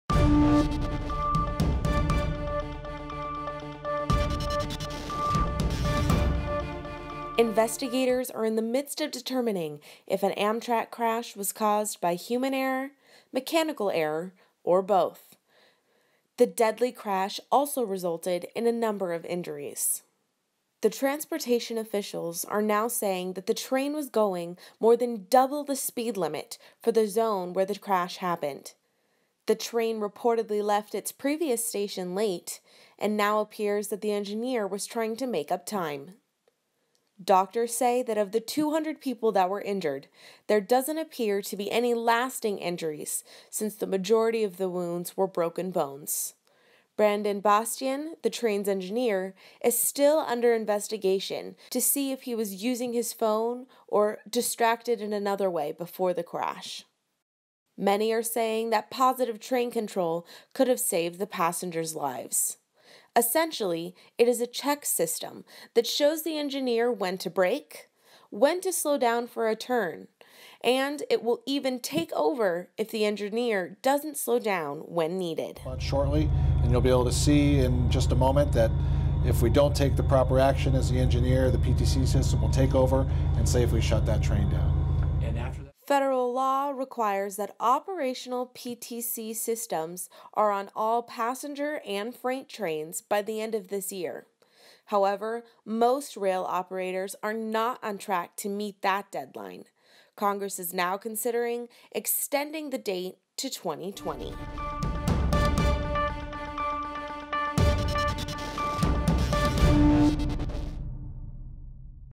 See What Technology Could Have Prevented Deadly Amtrak Crash
Supporters of the Positive Train Control systems are saying it could have saved lives in this weeks Amtrak Crash. With nearly 10 dead and over 200 injured the Amtrak engineer is now under investigation.